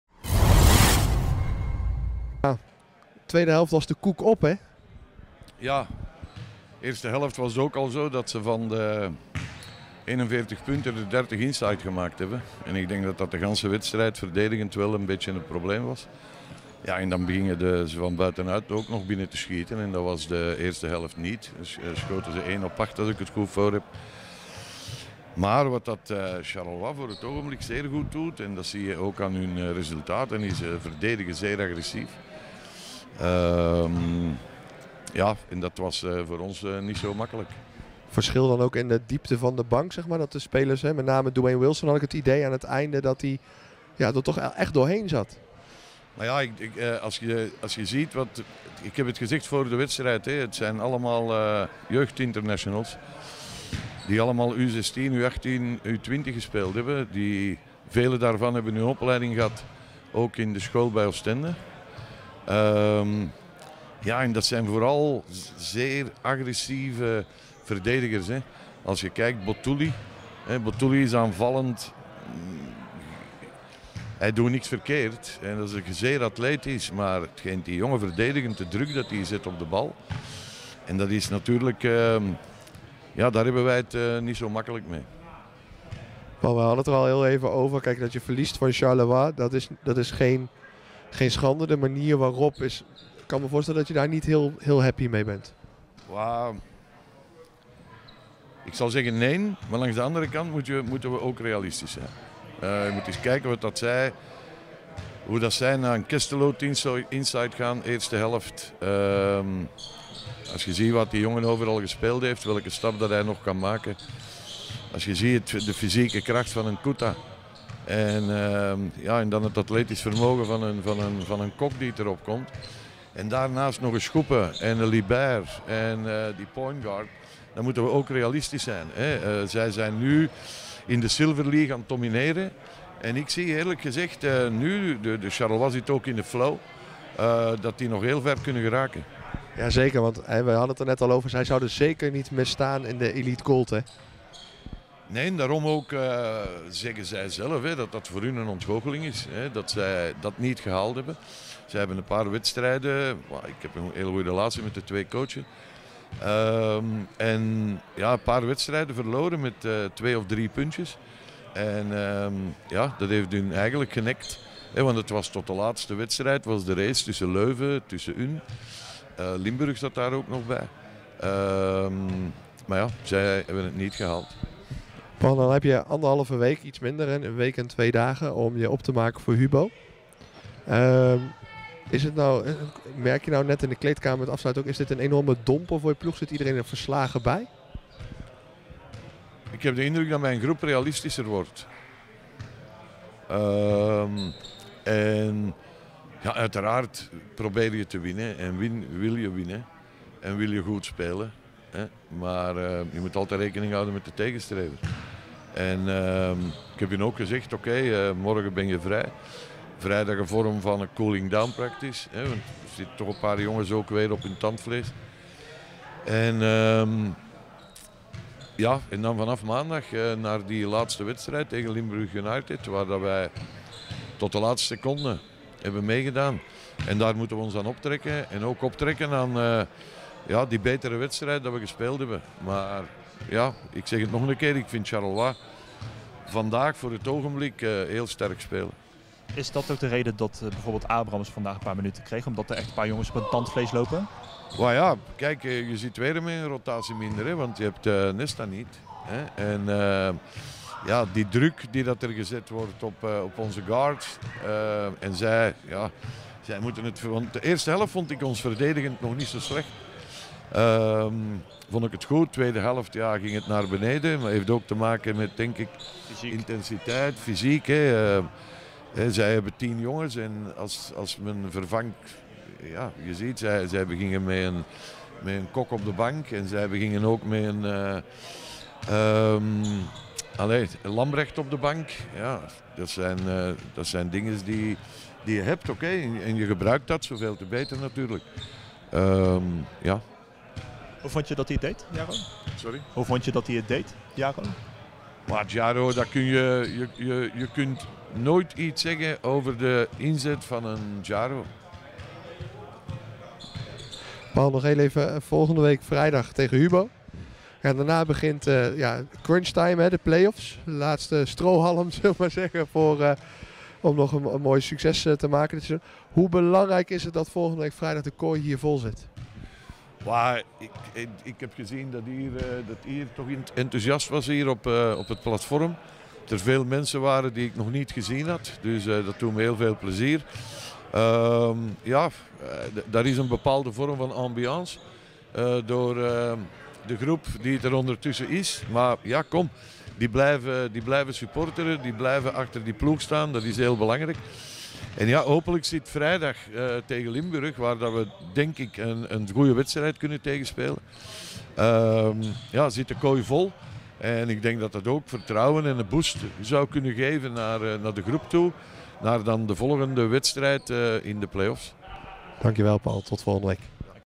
Paul Vervaeck: "Mijn groep wordt steeds realistischer en we moeten ons opladen voor Limburg United."
Na afloop van de verloren voorlaatste wedstrijd tegen Spirou Basket Charleroi spraken Sam Linders en Steven Teunissen met coach Paul Vervaeck van Yoast United. Hoe gaat hij zijn ploeg verder voorbereiden voor de komende Playoffs.